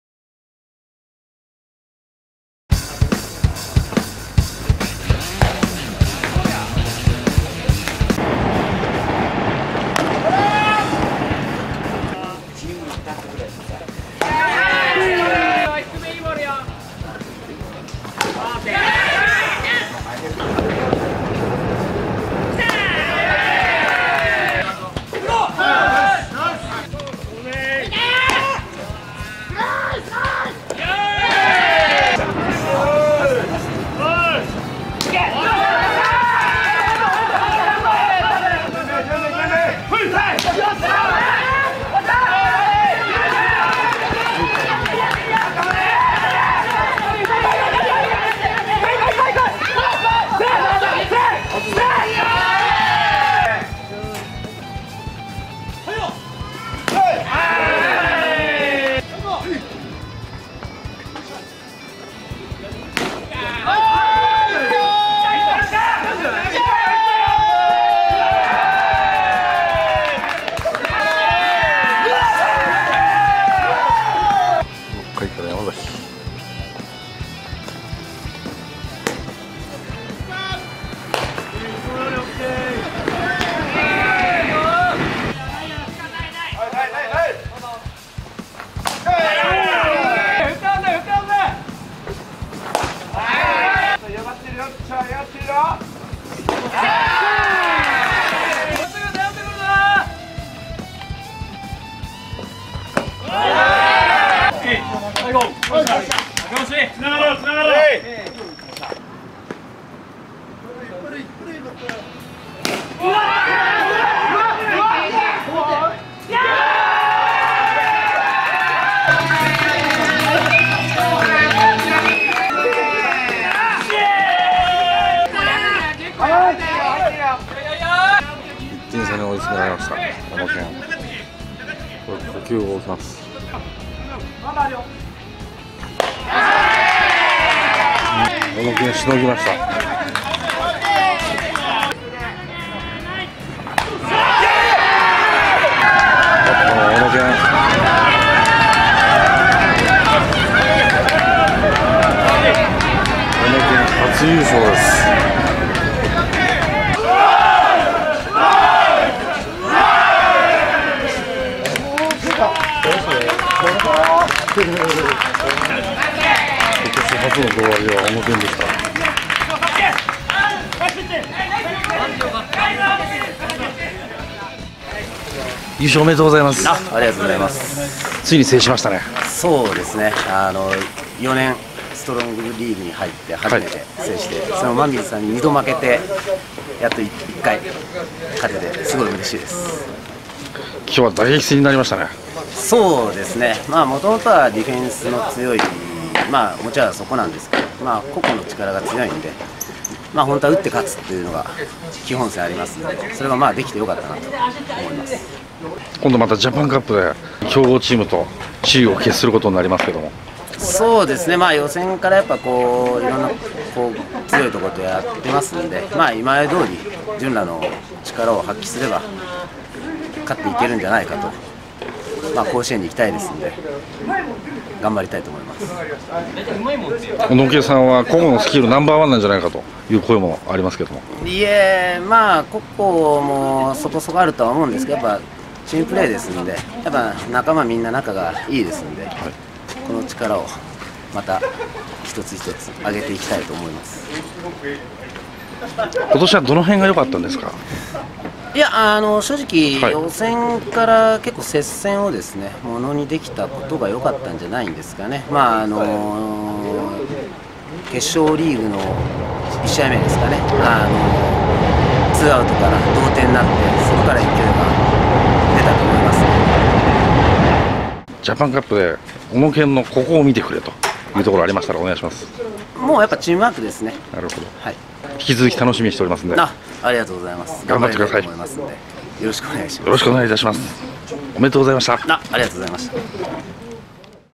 インタビュー 小野建ライオンズ（千葉）ジャパンカップ2015東日本1部
参戦4年目でストロングカップ2015（春）関東特別部を初制覇！悲願のジャパンカップ出場を決めた小野建ライオンズ。投打に個々のスキルが高く優勝候補の呼び声も高いが「チームワーク」で初出場初Ｖを目指す！今村剛主将インタビュー！